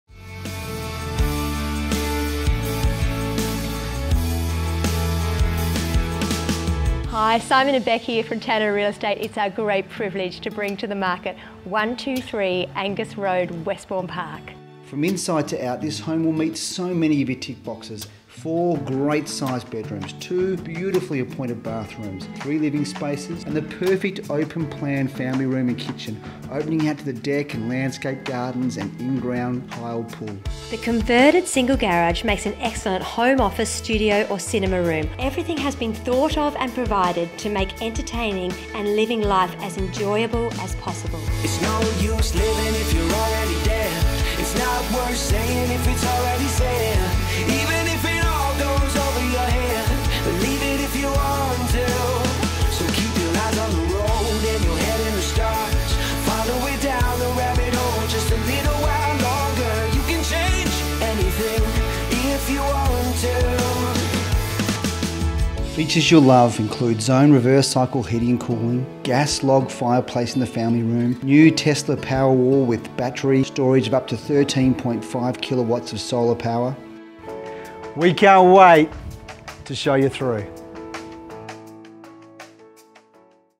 123 Angas Rd Westbourne Park
123 Angas Rd Westbourne Park, the ultimate in lifestyle and Location. Tesla Solar Battery Storage, Tiles and Heated inground pool with glass fencing,  four bedrooms, three living and built-in home office. Auto gates with offstreet parking and the ultmate man cave or studio. Tennis courts and palyground across the road. Close to Scotch College, Mercedes College, Westbourne Park Primary.